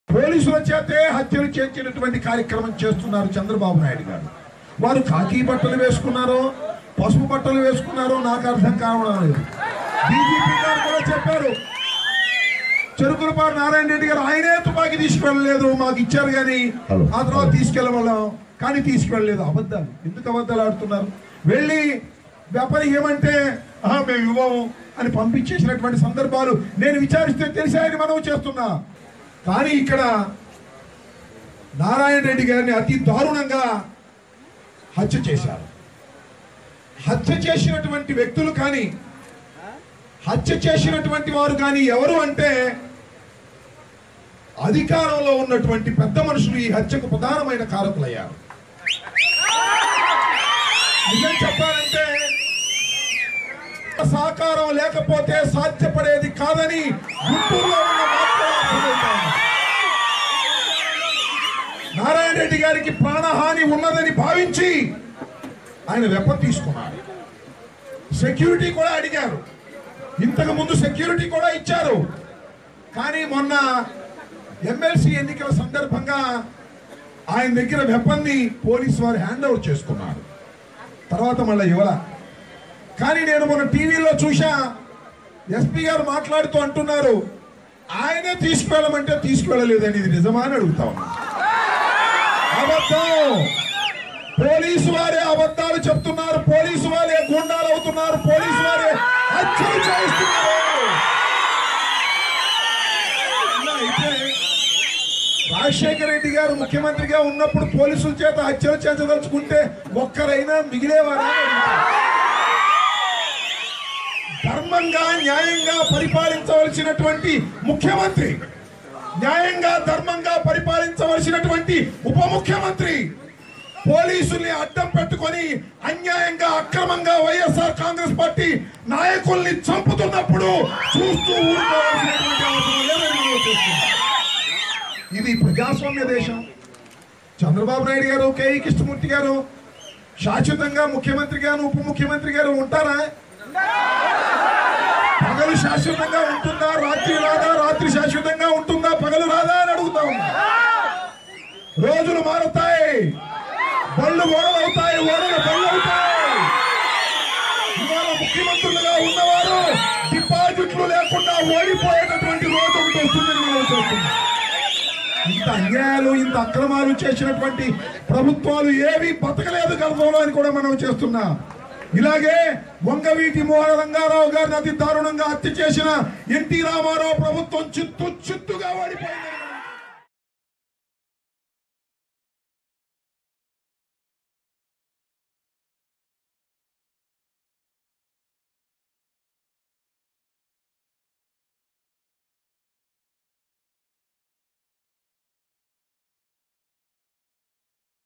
Ambati Ram Babu Fires On AP CM Chandrababu Over YSRCP Narayana Reddy Case | Mango News
Amabti Rambabu fired on AP Cm Chandra Babu Naidu and also deputy cm for the taking life of the YSRCP leader Narayana Reddy as he was slayed very brutally,.